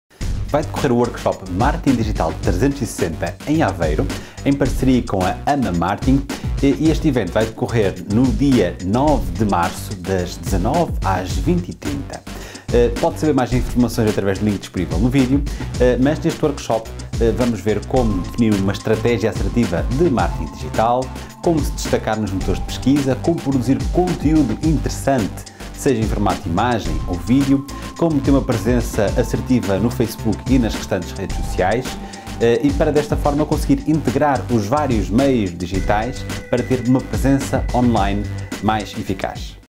marketing digital 360 aveiro
Workshop Marketing Digital 360 em Aveiro com a AMA Empresarial - Associação de Marketing e Atitude Empresarial​ já no dia 9 de março às 19h